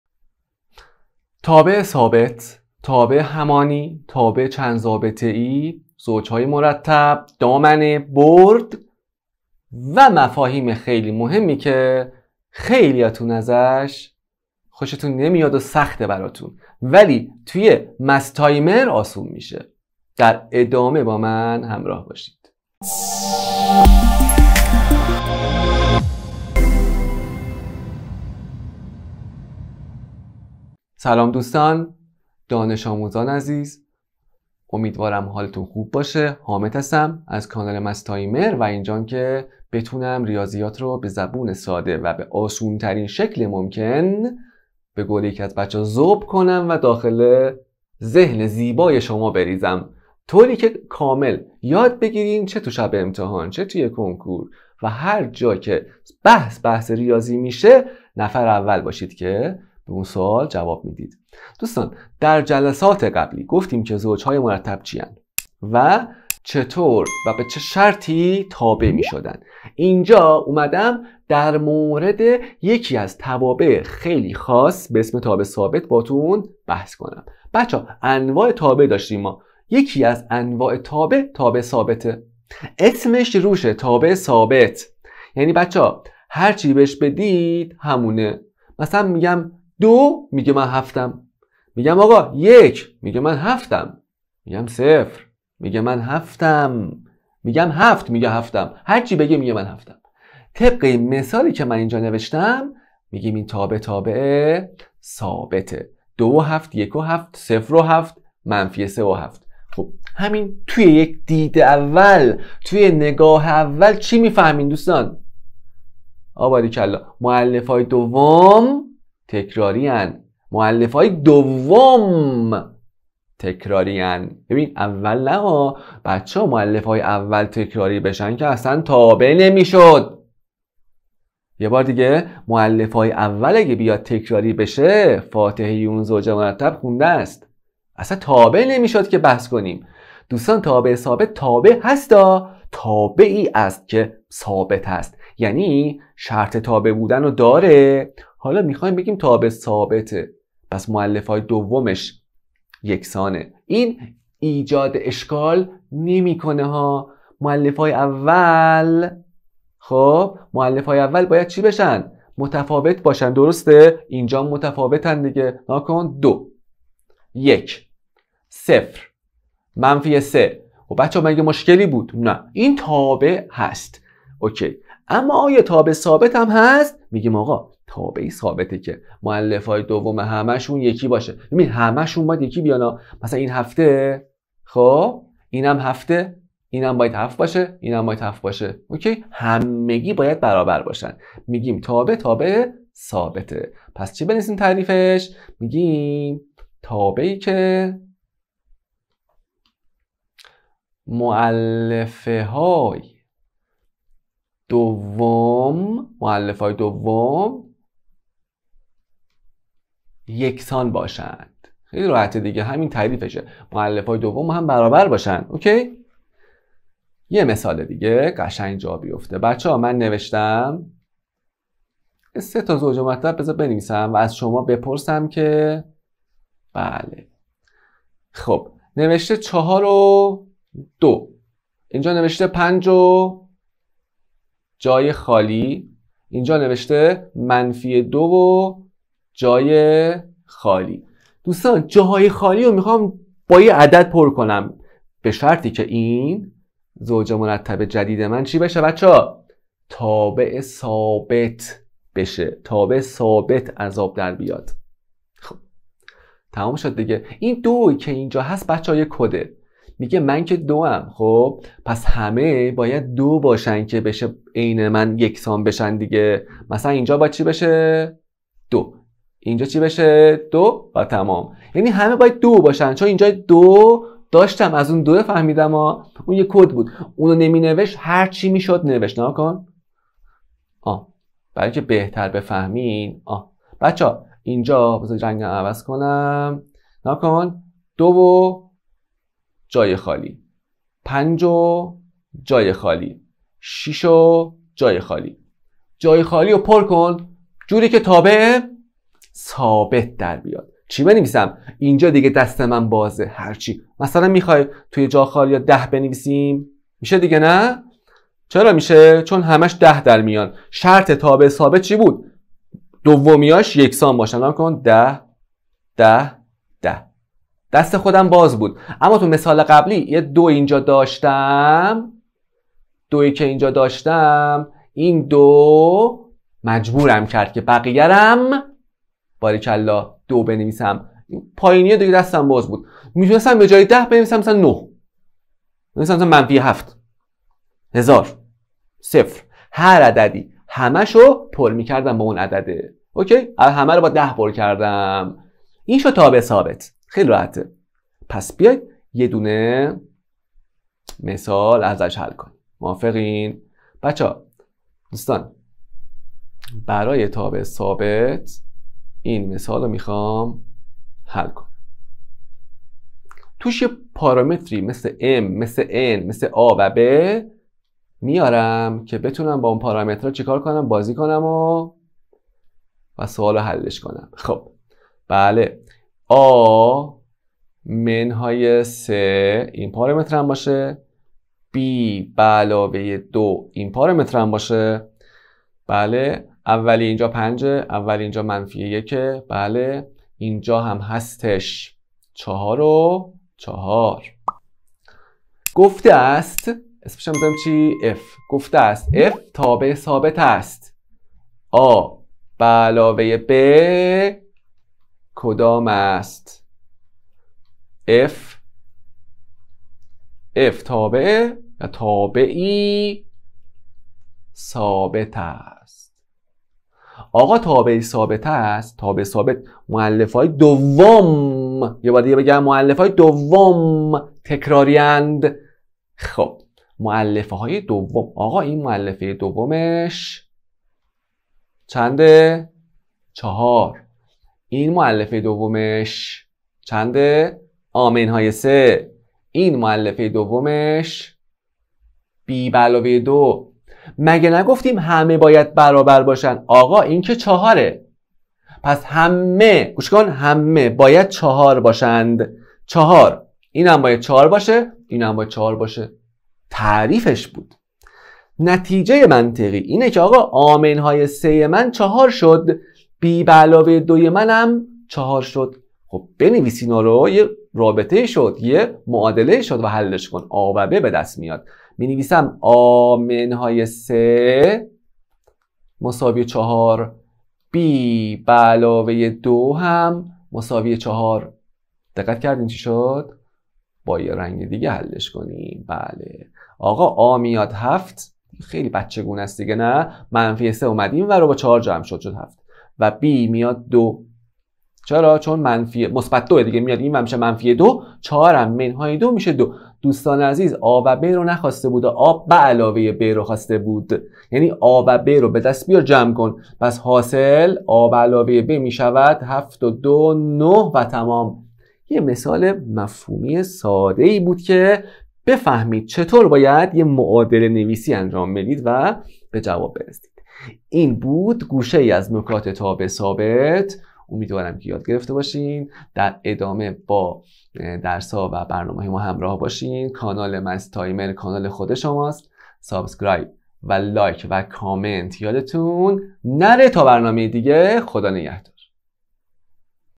تابع ثابت (حامد شهبانی)
در این ویدئو می آموزید که در چه شرایطی یک تابع ، تابع ثابت است و یا به عبارتی در چه شرایطی زوج های مرتبط تشکیل یک تابع ثابت می دهند
با مستایمر همراه شوید
mathtimer
تدوین : عبداله محمدی
 زیرنویس : پویا بختیاری